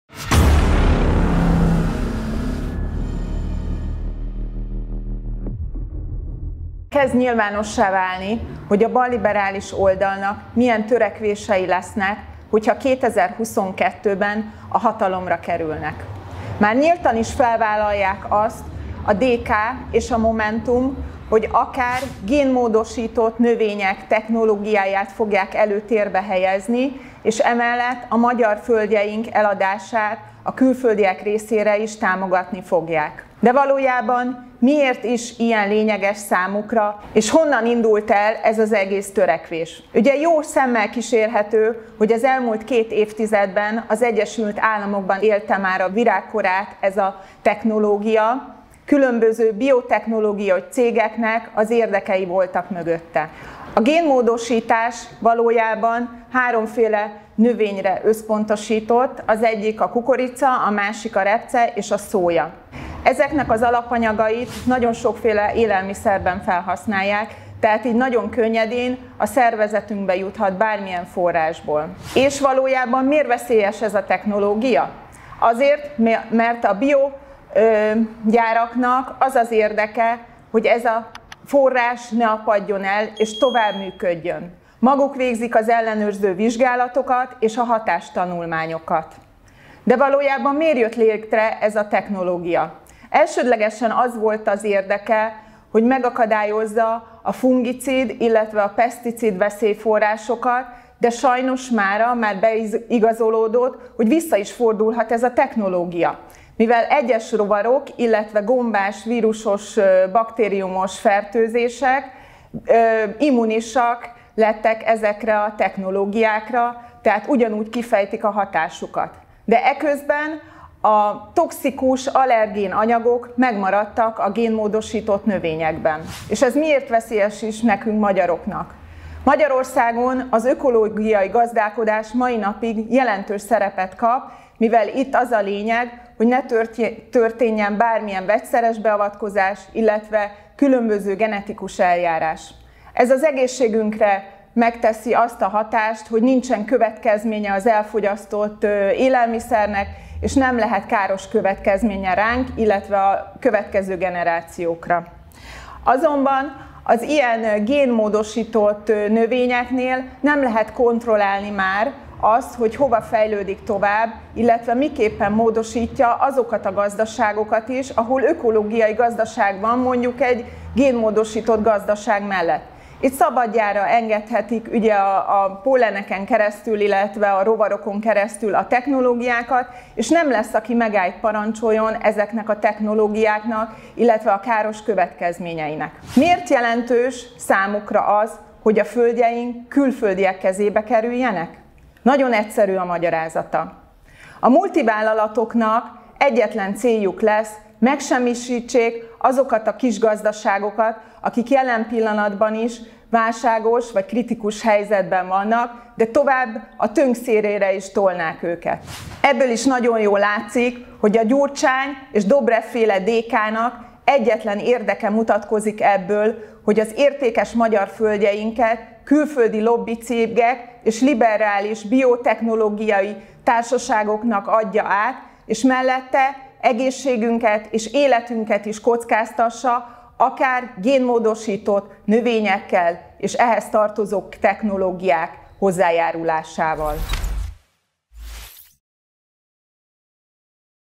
Megvalósulhat a balliberális métely
A gyurcsányista érdekszövetség hatalomra kerülésével nemcsak az értékes magyar földjeink, de még az egészséges életünk is áldozatul eshet a nemzetközi pénzvilág törekvéseinek. A DK és a Momentum már a programjukban is céloz arra, hogy miképpen fogják előtérbe helyezni a nyugati géntechnológiák alkalmazását és a földterületek külföldiek felé történő értékesítésének megkönnyítését, amelynek a magyar gazdák is végzetes elszenvedői lehetnek.